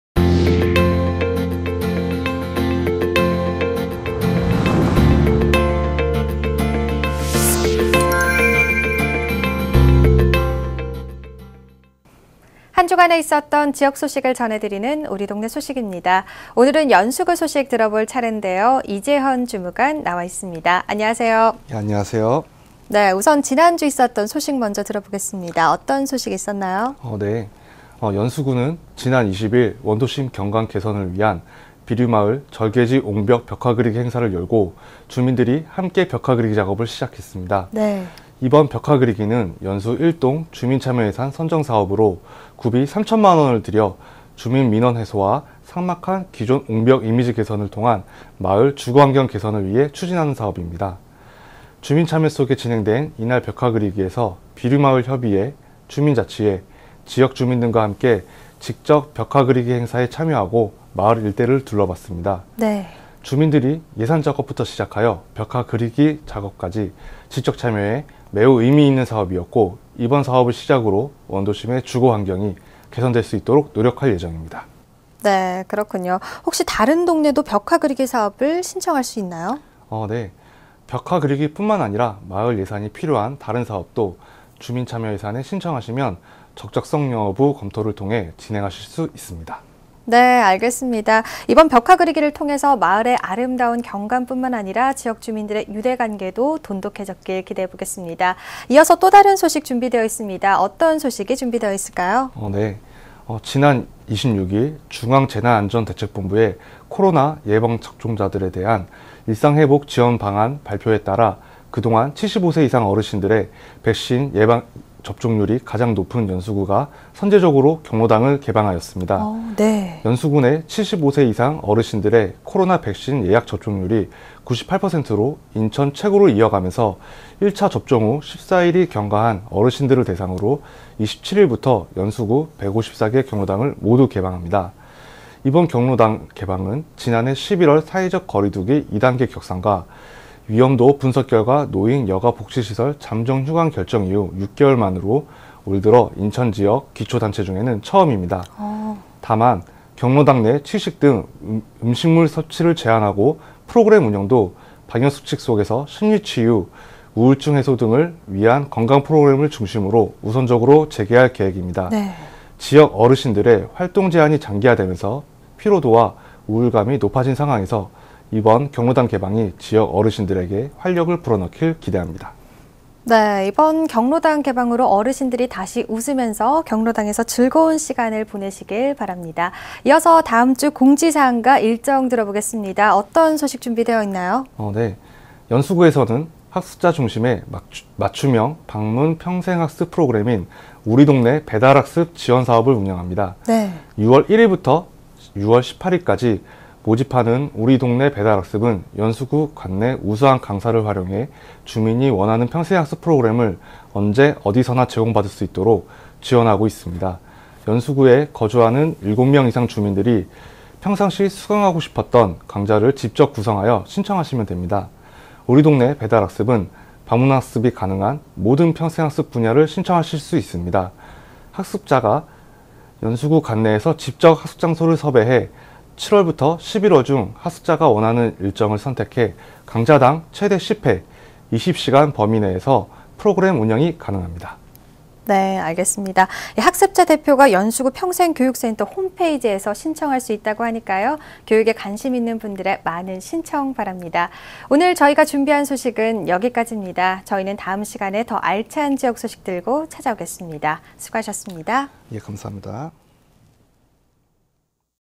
[NIB 뉴스] 우리 동네 소식-6월04일 연수구
(오프닝)한 주간의 있었던 지역 소식을 전해드리는 ‘우리 동네 소식’입니다. 오늘은 연수구 소식 들어보겠습니다. 연수구 이재현 주무관 나와 있습니다. 안녕하세요.

(앵커) 우선 지난 주 있었던 소식 먼저 들어보겠습니다. 어떤 소식 준비 돼 있나요?

 이재현 / 연수구 주무관
(이재현) 연수구는 지난 20일 원도심 경관 개선을 위한 비류마을 절개지 옹벽 벽화 그리기 행사를 열고, 주민들이 함께 벽화 그리기 작업을 했습니다.

CG1. 연수구, 지난 20일 ‘비류마을 절개지 옹벽 벽화 그리기’ 진행
이번 벽화 그리기는 연수1동 주민참여예산 선정 사업으로 구비 3천만 원을 들여 주민 민원 해소와 삭막한 기존 옹벽 이미지 개선을 통한 마을 주거환경 개선을 위해 추진하는 사업입니다.

주민참여 속에 진행된 이 날 벽화 그리기에서 비류마을 협의회, 주민자치회, 지역주민 등과 함께 직접 벽화 그리기 행사에 참여하고 마을 일대를 둘러봤습니다.

주민들이 예산작업부터 시작하여 벽화 그리기까지 직접 참여해 매우 의미 있는 사업이었고, 이번 사업을 시작으로 원도심의 주거환경이 개선될 수 있도록 노력할 예정입니다.

(앵커) 그렇군요. 다른 동네도 벽화 그리기 사업을 신청할 수 있나요?

(이재현)네. 벽화 그리기뿐만 아니라 마을에 예산이 필요한 다른 사업도 주민참여예산에 신청하시면 적정성 여부 검토를 통해 진행하실 수 있습니다.

(앵커) 네. 알겠습니다. 이번 벽화 그리기를 통해 마을의 아름다운 경관뿐만 아니라, 주민들의 유대관계도 깊게 형성됐길 기대해봅니다. 이어서 다음소식 들어보겠습니다.

(이재현) 지난 26일 중앙재난안전대책본부의 코로나 예방 접종자들에 대한 일상 회복 지원방안 발표에 따라 그동안 75세 이상 어르신들의 백신 예약 접종률이 가장 높은 연수구가 선제적으로 경로당을 개방하였습니다.

CG2. 연수구, 백신접종 어르신에 지역 154개 경로당 전면 개방
연수구 내 75세 이상 어르신들의 코로나19 백신 예약 접종률이 98%로 인천 최고를 이어가면서 1차 접종 후 14일이 경과한 어르신들을 대상으로 27일부터 연수구 154개 경로당을 모두 개방합니다.

이번 경로당 개방은 지난해 11월 사회적 거리두기 2단계 격상과 위험도 분석 결과 노인여가복지시설 잠정 휴관 결정 이후 6개월 만으로 올해 들어 인천지역 기초단체 중에는 처음입니다.

CG3. 경로당 음식물 섭취 제한 및 방역수칙 속 프로그램 운영
다만 경로당 내 취식 등 음식물 섭취를 제한하고 프로그램 운영도 방역수칙 속에서 심리 치유, 우울증 해소 등을 위한 건강프로그램을 중심으로 우선적으로 재개할 계획입니다.

지역 어르신들의 활동 제한이 장기화하면서 피로도와 우울감이 높아진 상황에서, 이번 경로당 개방이 지역 어르신들에게 활력을 불어넣길 기대합니다.

(앵커) 네 이번 경로당 개방으로 어르신들이 다시 모여 웃으며 즐거운 시간을 보내시기 바랍니다. 이어서 다음 주 일정과 공지 사항 들어보겠습니다. 어떤 소식이 있나요?

(이재현) 연수구에서는 학습자 중심의 맞춤형 방문 평생학습 프로그램인 ‘우리동네 배달학습’ 지원 사업을 운영합니다.

CG4. 연수구, 6월부터 ‘우리 동네 배달학습’ 지원 사업 진행
6월 1일부터 6월 18일까지 모집하는 ‘우리 동네 배달학습’은 연수구 관내 우수한 강사를 활용해 주민이 원하는 평생학습 프로그램을 언제, 어디서나 제공 받을 수 있도록 지원하고 있습니다. 연수구에 거주하는 7명 이상 주민들이 평상시 수강하고 싶었던 강좌를 직접 구성하여 신청하시면 됩니다.

CG5. ‘우리 동네 배달학습’ 방문 가능한 평생학습 분야 신청 가능
우리 동네 배달학습’ 은 방문학습이 가능한 모든 평생학습 분야를 신청할 수 있습니다. 학습자가 연수구 관내에서 직접 학습장소를 섭외해 7월~11월 중 학습자가 원하는 일정을 선택해, 강좌 당 최대 10회, 20시간 범위에서 프로그램 운영이 가능합니다.

(클로징) 네. 학습자 대표가 연수구 평생교육센터 홈페이지에서 신청할 수 있다고 하니까요. 관심 있는 분들의 많은 신청 바랍니다. 오늘 준비한 소식은 여기까집니다. 다음 시간에 더 알찬 지역 소식 들고 찾아오겠습니다. 수고하셨습니다.